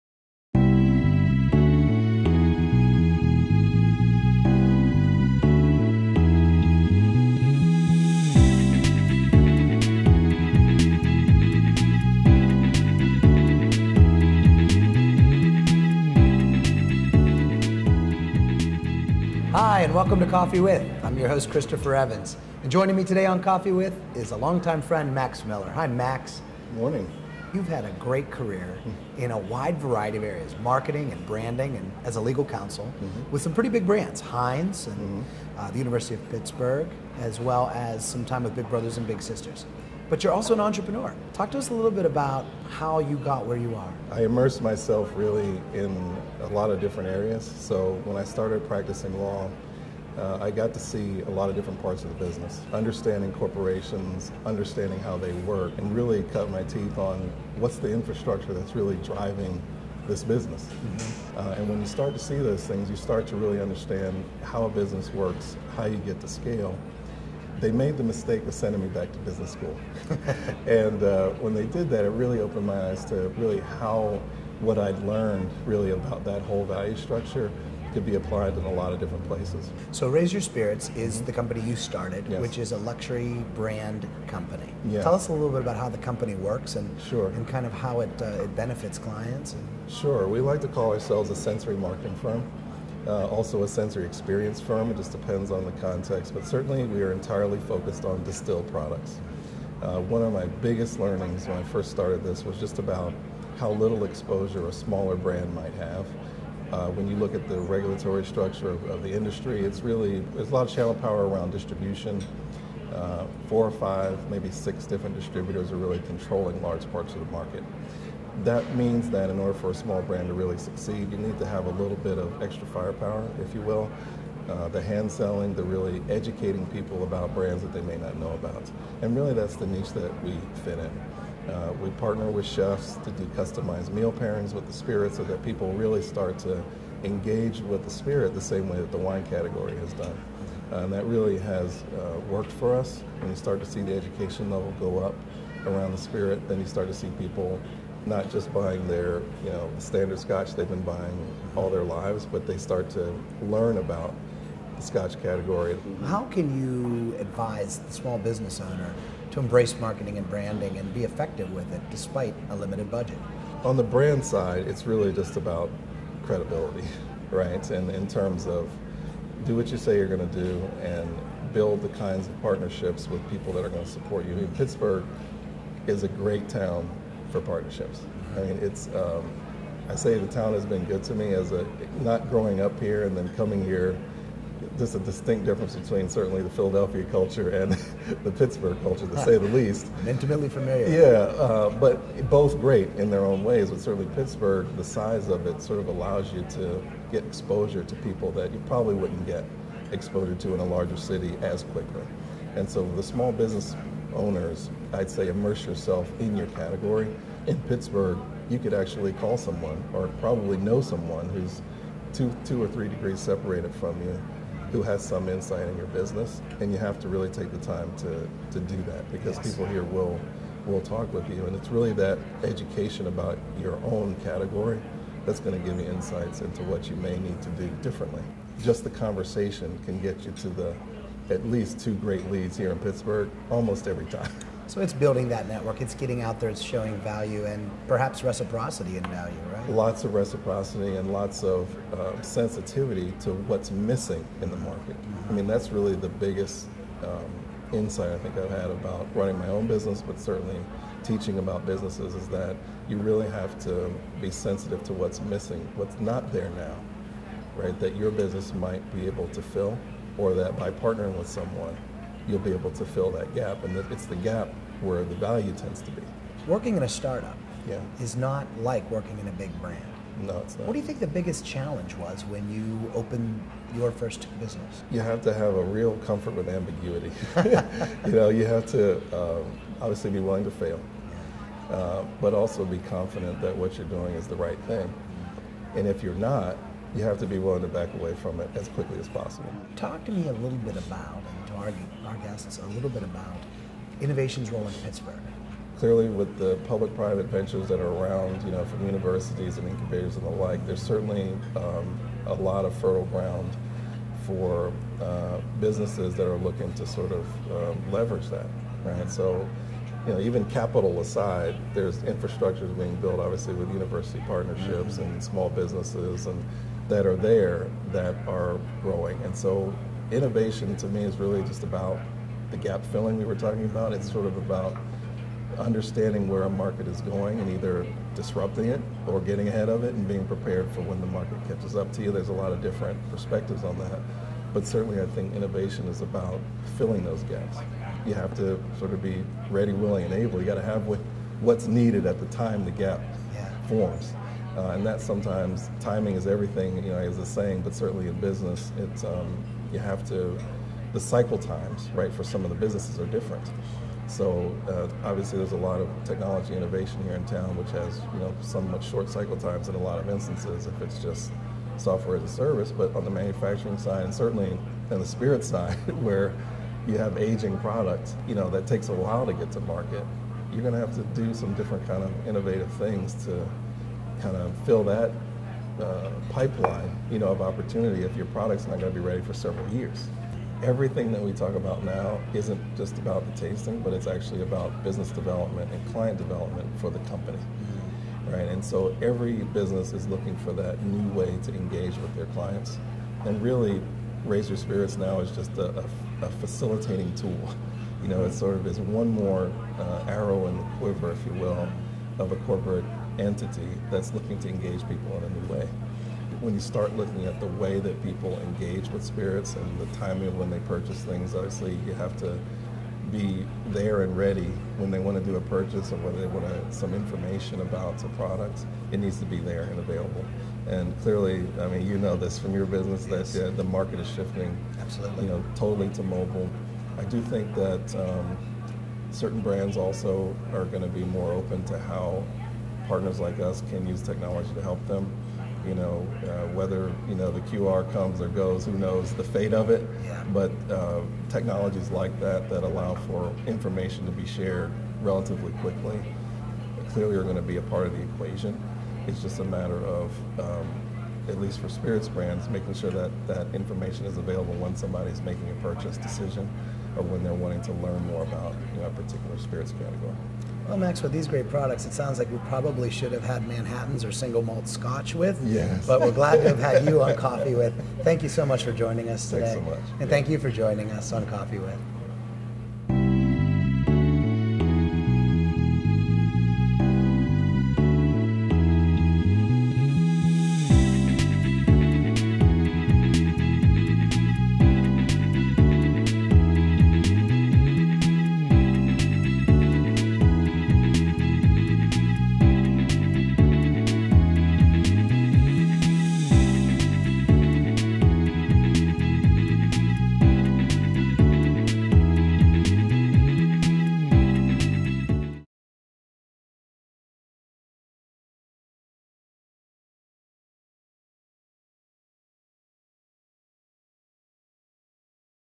Coffee With Max Miller Entrepreneur, Educator, Attorney
A chat with Max Miller. Max Miller has 20 years of business experience in roles as a corporate attorney, entrepreneur, non-profit leader, and educator.  He is currently the Director of VIP Experiences for MetroMe -- a premier smartphone concierge application, and the Managing Director and Founder of Raise Your Spirits, offering fine craft spirits for those with discerning tastes.   Most recently  Max served as the Chief Administrative Officer for Big Brothers Big Sisters of America's national office, and prior to that served as the founding director of the Innovation Practice Institute at Pitt Law. Max cut his teeth in the corporate world as an attorney/ marketer  for Heinz (7 years) and as an attorney for Federated Investors (4 years).